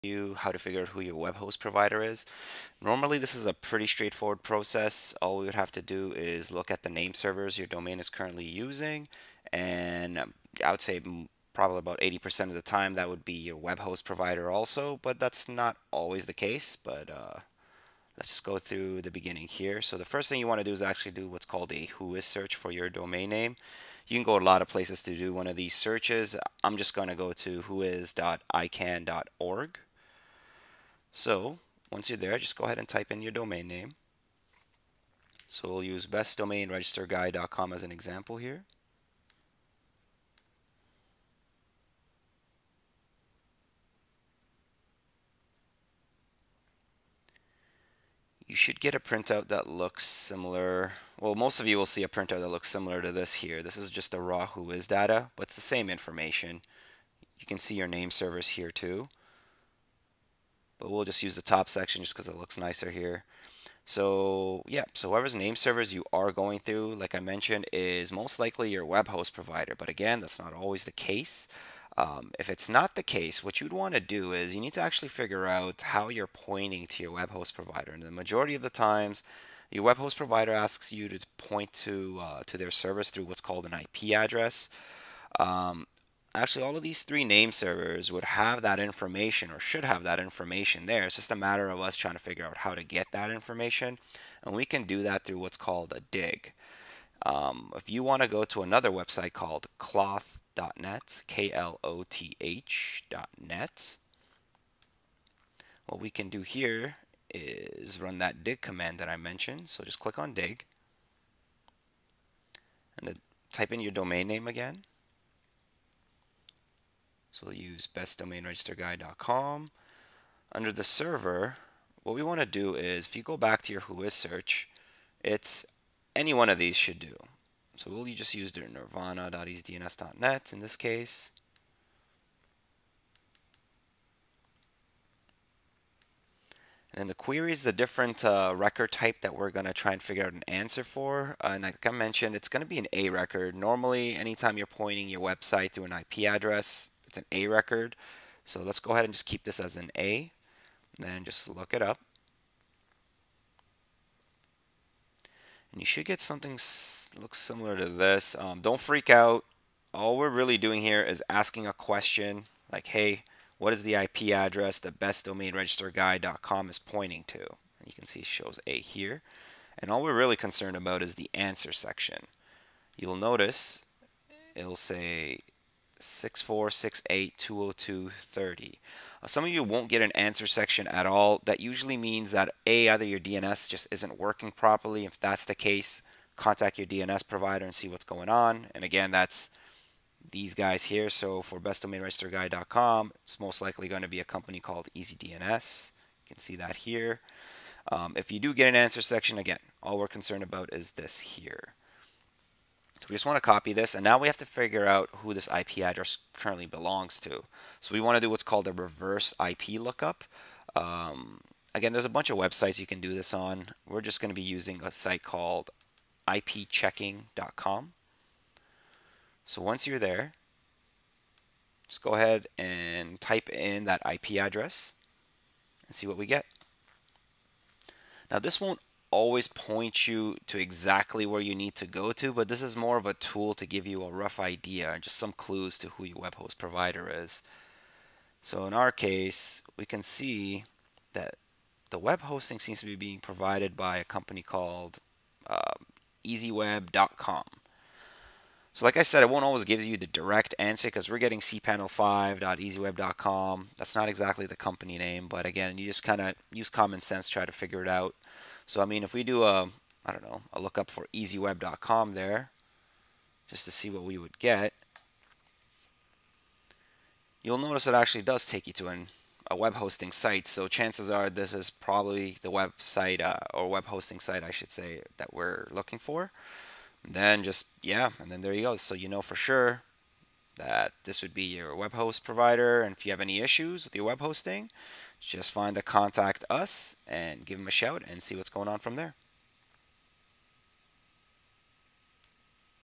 How To Find Your Web Host Provider
Many people split their domain's DNS and web hosting services with two companies and then forget which service is with whom.

This 'How To' will walk you through how to figure out who your providers are.